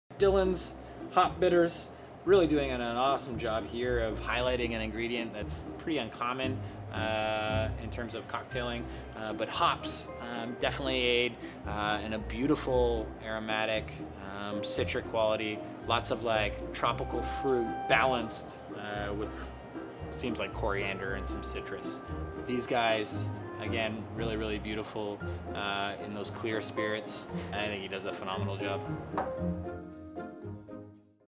Dillons Hops Bitters
The Brewmaster’s bestie.

Should I order a beer? Or maybe a cocktail? Beer? Cocktail? The struggle is real. But really, why settle for a derivative brew when you can infuse your cocktails with beer-y blast? Blending a combination of local hops varietals, including Cascade, Mount Hood and Alpha Aroma, these bitters lend brew mastery to mixology. High citrus and pine notes deepen the flavour profile of cocktails, ciders and even…drumroll please…beers.

Handcrafted one batch at a time in Beamsville, Ontario, by a father and son pair, Dillon’s proves that you can turn your passion into a family affair. So next time a regular ole brew won’t do, end the struggle and add a drop or two.